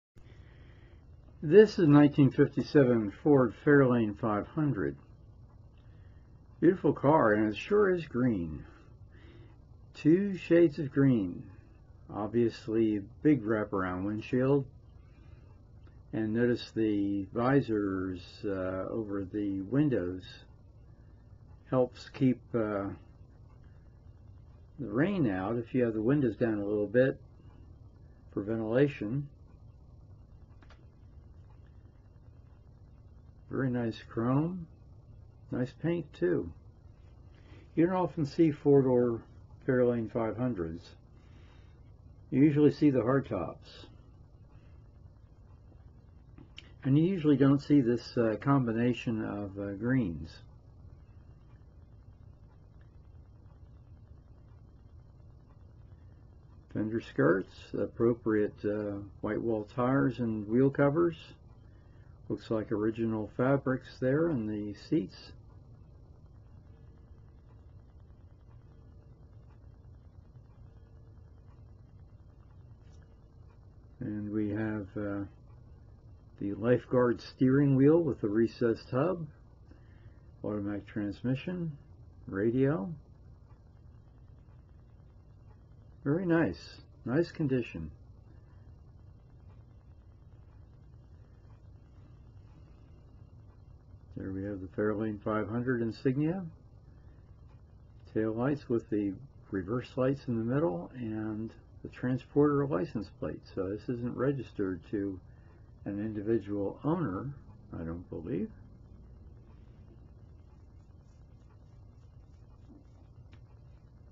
1957 Ford Fairlane 500 Four Door Sedan Grn TV1218215980
Ford really had lots of style lines for '57...four to be exact, something for everyone.  Nice styling that incorporated modest fins and very nice two tone color combinations available.
Show less